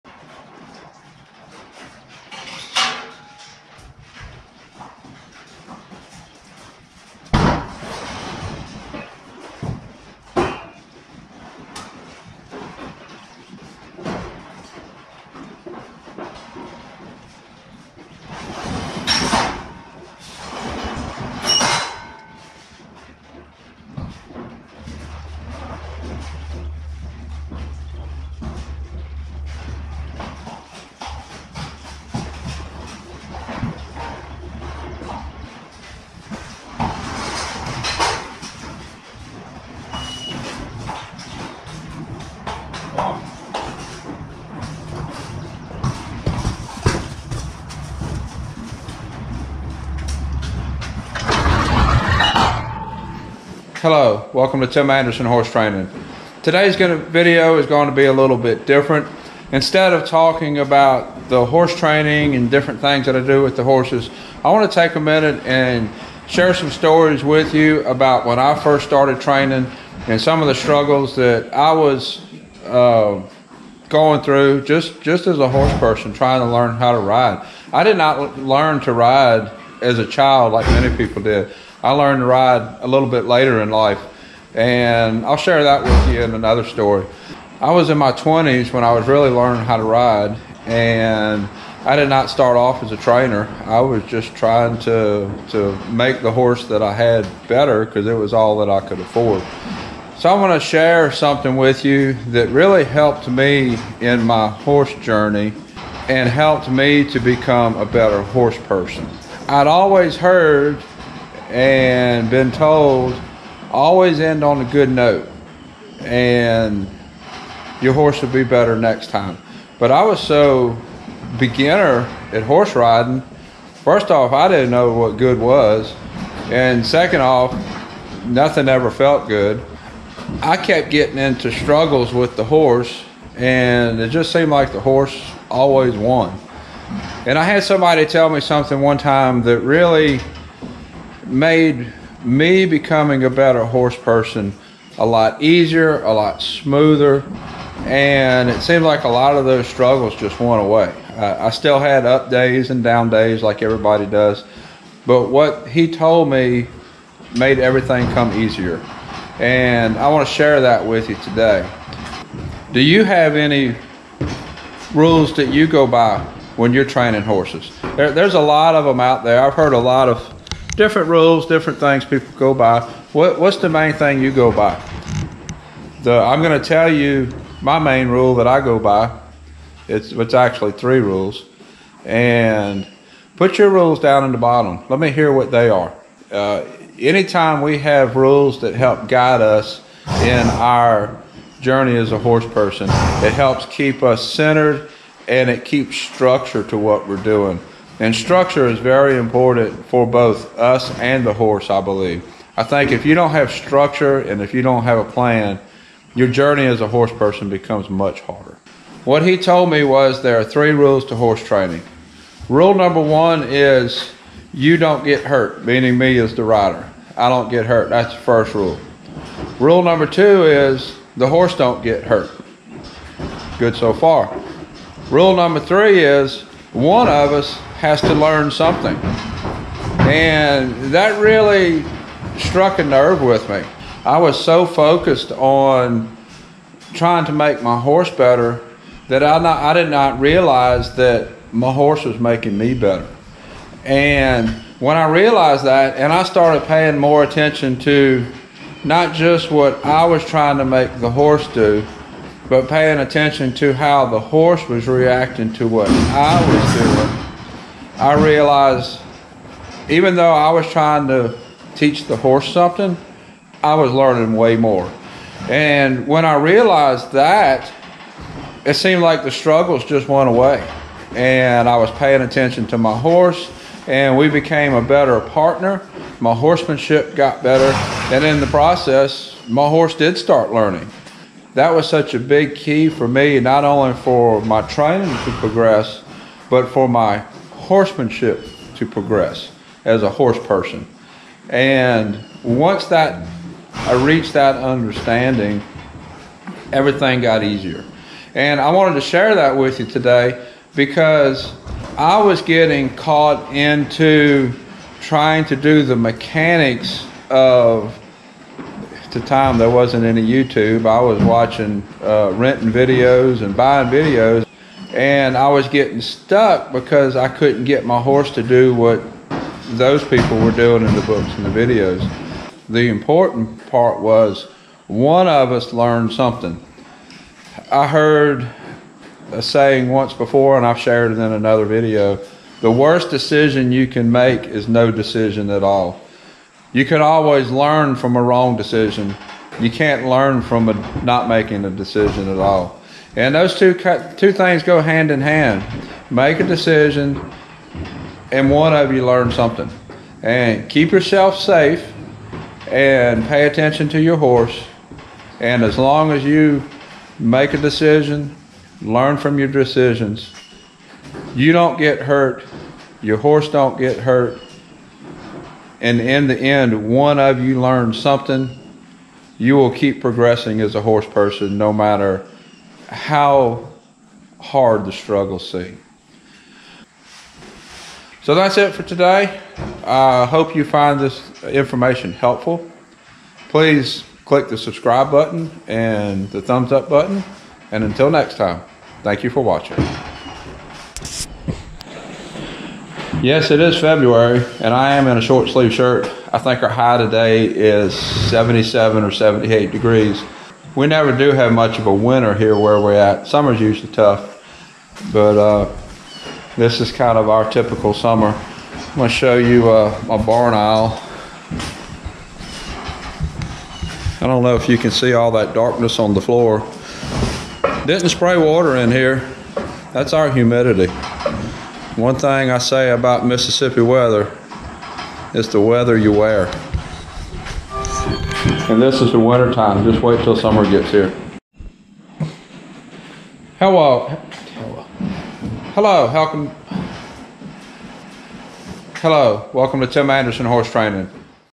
Transforming Struggles into Skills in Horse Training
Becoming a better horse rider takes a lot of dedication and hard work. In this video I give you the one piece of advice that was a game change in helping me to become a better horse rider.

Tim Anderson
Ocean Springs, MS 39566